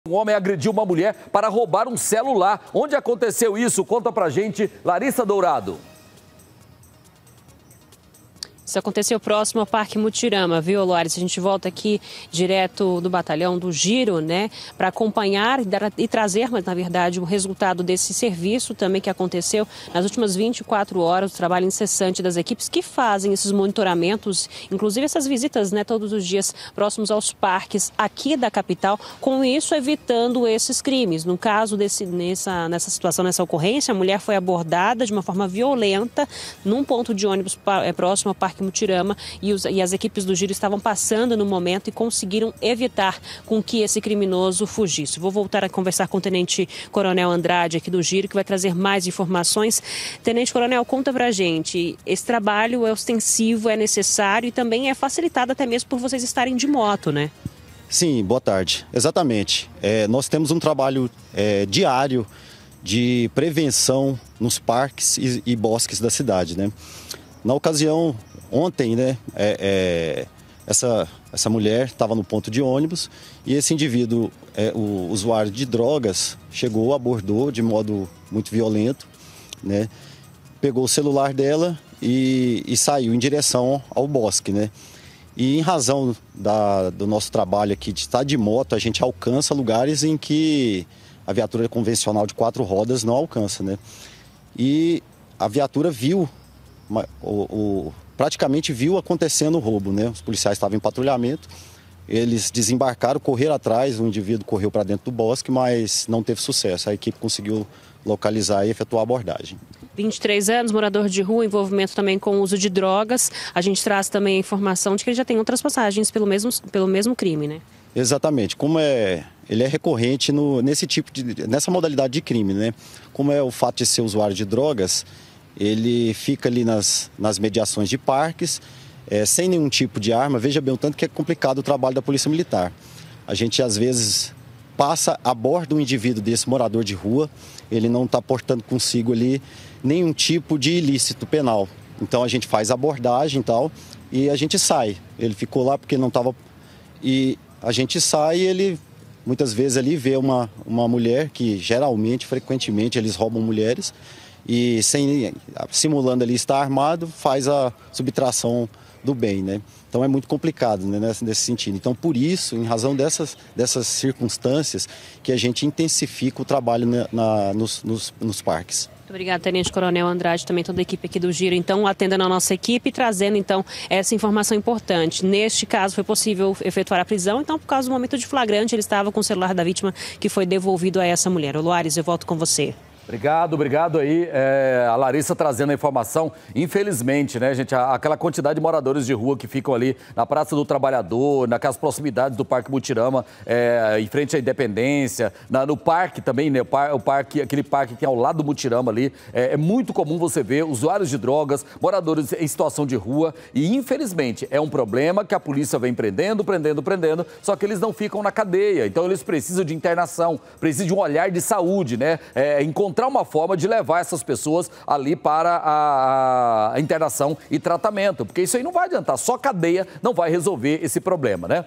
BG 100724 LINK LARISSAROUBO MULHER MUTIRAMA ROUBO DE CELULAR
ROUBO DE CELULAR: MULHER É AGREDIDA NO PARQUE MUTIRAMA POR BANDIDO.